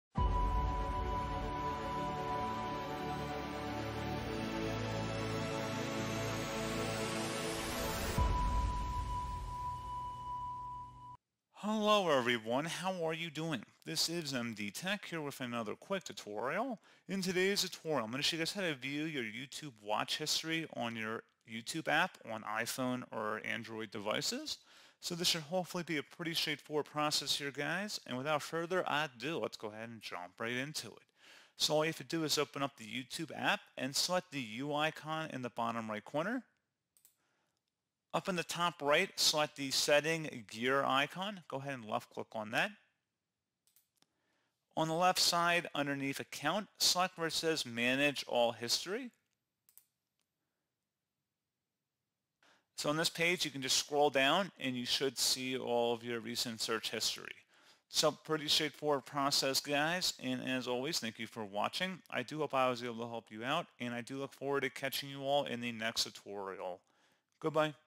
How To View YouTube Watch History [Tutorial]
This tutorial explains how to view your YouTube watch history, allowing you to easily track the videos you've watched. By accessing your account settings, you can see a chronological list of all the content you've previously viewed, making it simple to revisit your favorite videos. Viewing your watch history is helpful for users who want to explore past content or manage their recommendations. This feature also gives you insight into your viewing patterns, helping to optimize your YouTube experience.

This tutorial will apply for iOS (iPhone, iPad), Android, and Windows devices running the YouTube  app.